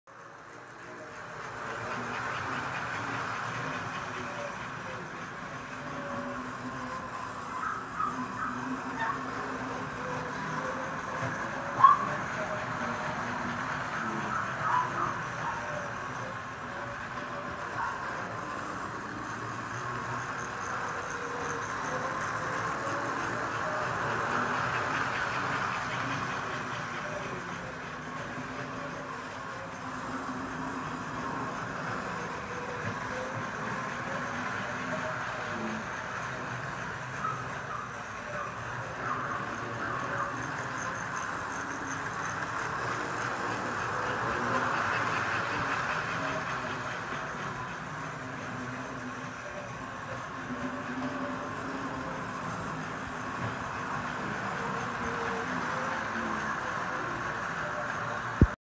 1951 Gilbert American Flyer Northern 332AC After Repair
1951 Gilbert American Flyer Northern 332AC running on the layout after repair.